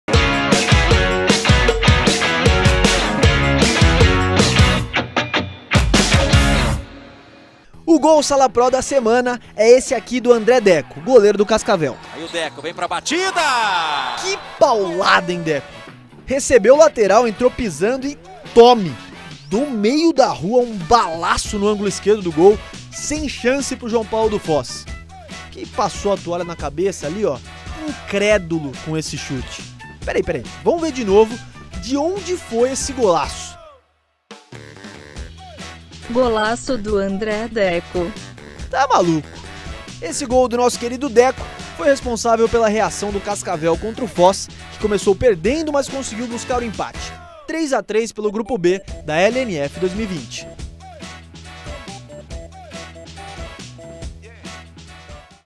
LNF2020 - Gol da Semana - Sala Pro Umbro
André Deko, goleiro do Cascavel, fez o Gol mais bonito da Semana na LNF2020